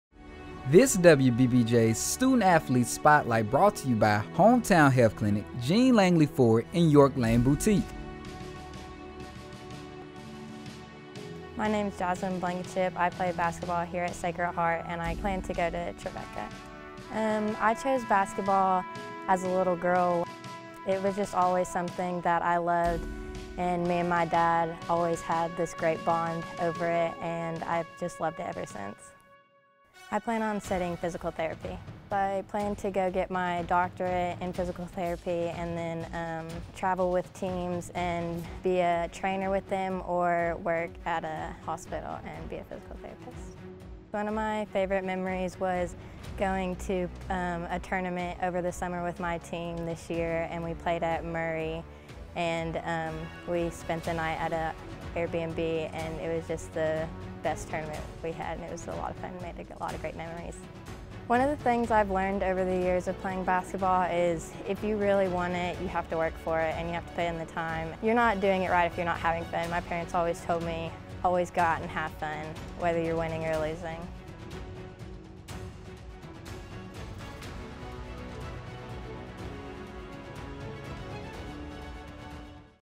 National Student-Athlete Day | Jasmine Blankenship, Sacred Heart
Join us in honoring Student-Athlete Jasmine Blankenship from Sacred Heart High School.

It takes a special person, one talented both on and off the field, to overcome the challenges that forge a college-level athlete. April 6th is NationalStudentAthleteDay and we are highlighting high school seniors choosing to continue their sports careers into college.